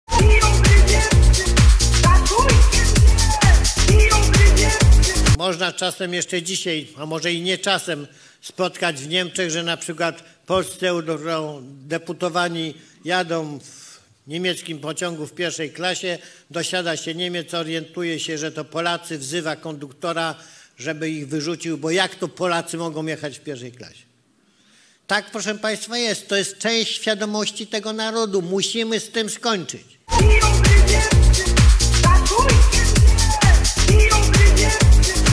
Odlot Kaczyńskiego! Biją mnie! Niemcy wyrzucają Polaków z pociągów 1 klasy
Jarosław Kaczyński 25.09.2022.
Polacy jadą w niemieckim pociągu w pierwszej klasie, dosiada się Niemiec, orientuje się że to Polacy, wzywa konduktora żeby ich wyrzucił. Bo jak to Polacy mogą jechać w 1 klasie? Tak jest. To jest część świadomości tego narodu!

— To jest dokładnie to samo, co można czasem jeszcze dzisiaj — a może i nie czasem — spotkać w Niemczech, że np. polscy eurodeputowani jadą w niemieckim pociągu w pierwszej klasie, dosiada się Niemiec, orientuje się, że to Polacy, wzywa konduktora, żeby ich wyrzucił, bo jak to Polacy mogą jechać w pierwszej klasie — opowiadał kilka dni temu na spotkaniu z mieszkańcami Opola Jarosław Kaczyński w kontekście reparacji, które Polska chce otrzymać od Niemiec.

O podobnej sytuacji, mającej być przykładem dyskryminacji Polaków za granicą opowiedział europoseł PiS Karol Karski. W rozmowie z Interią stwierdził, że na niemieckim lotnisku celowali w niego funkcjonariusze. W ten sposób chciał zapewnić o wiarygodności słów Jarosława Kaczyńskiego.